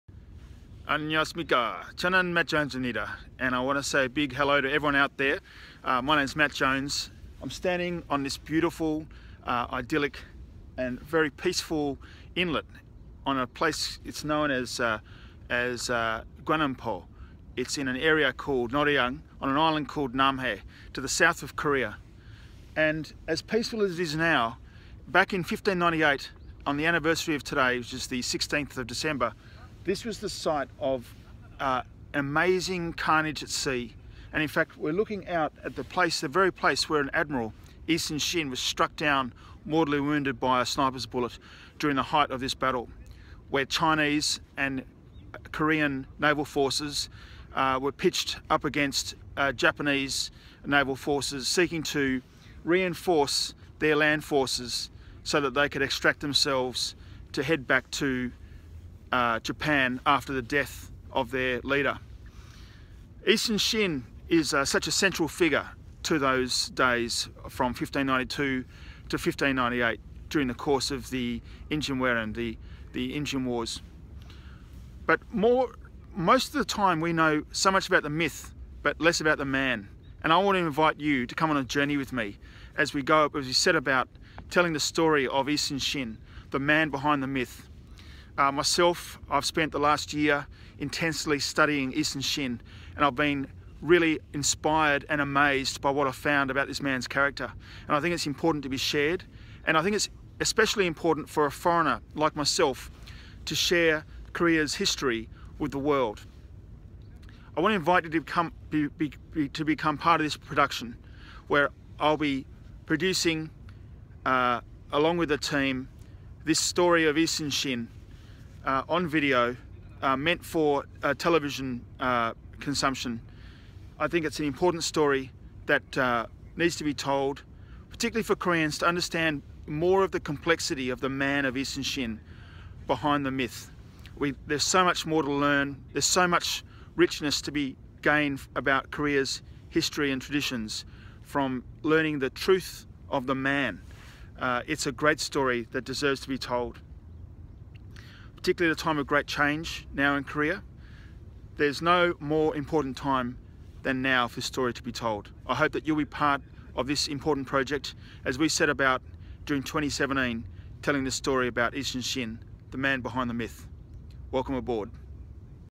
The Man Behind The Myth: Yi Sun-shin
Welcome Aboard! During 2017, I will be documenting a journey to discover the man behind the myth of Admiral Yi Sun-shin. The greatness of Yi Sun-shin is not in dispute, and everyone agrees that he is among the very finest naval commanders who ever lived. But much of what we know about The Admiral is embellished storytelling, and from that a myth emerges.
Through my studies of Korean history and Yi Sun-shin, I have found that the story of the actual man is far more compelling than the myth.
I'm inviting people to be partners in this endeavour, to allow a foreigner to tell a very important story in Korea's history for consumption by Westerners who have little knowledge of Korea or Yi Sun-shin.
I hope that you too will join this search as we go Looking For The Admiral.